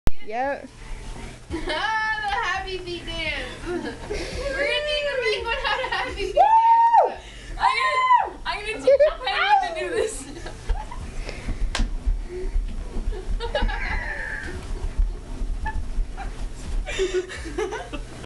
Happy Feet Dance
This is the happy feet dance created by Korianne and Carly...Drinking was involved!! lol I know its sideways too sorry about that